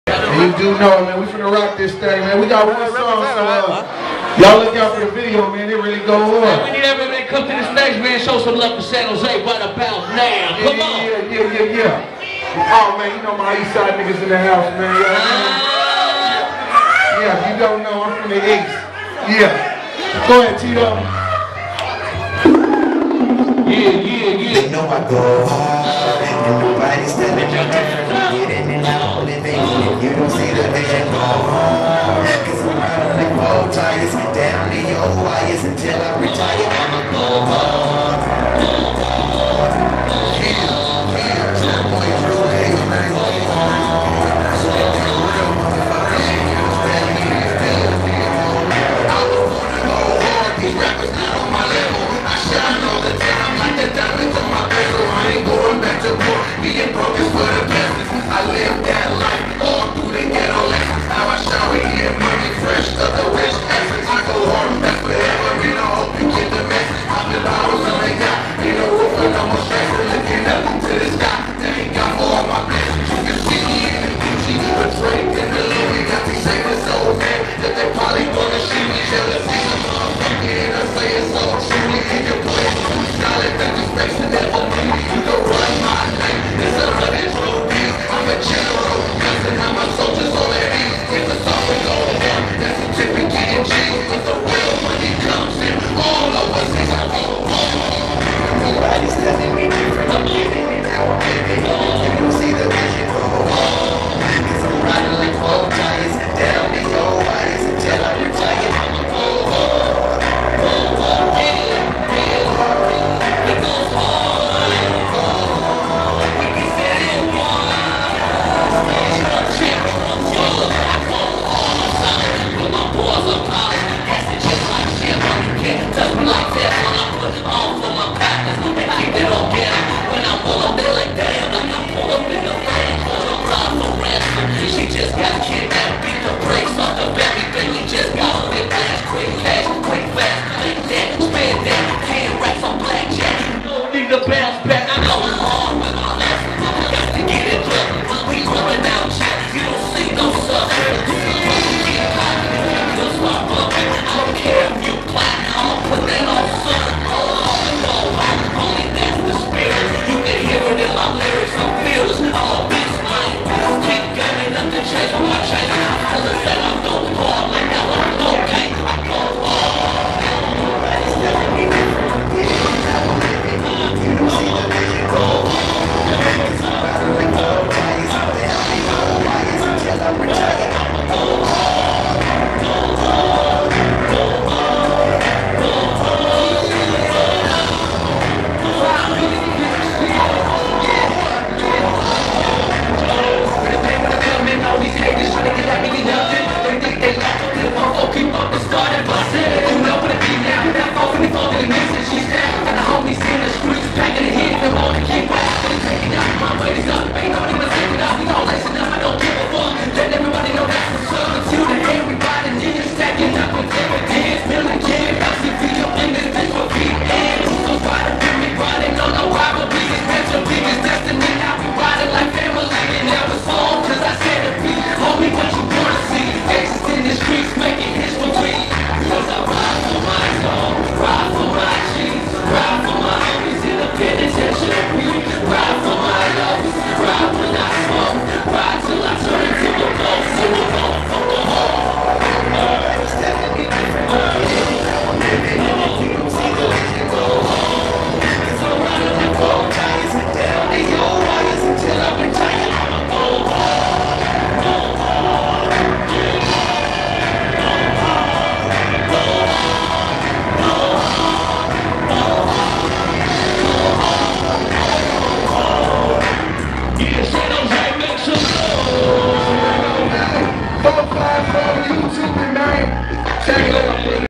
454 LIFE ENT DREW DEEZY, NUMP & THAI ROCKIN' THE SHOW IN SJ'S CLUB TOONS
454 LIFE ENT DREW DEEZY, NUMP & THAI ROCKIN' THE SHOW IN SJ'S CLUB TOONS. LITTLE PREVIEW OF WHAT'S MORE TO COME. THANK YOU FOR ALL THE LOVE AND SUPPORT. ASIANS/ISLANDERS 454 IS THE NEXT MOVEMENT, TAKING IT TO ANOTHER LEVEL.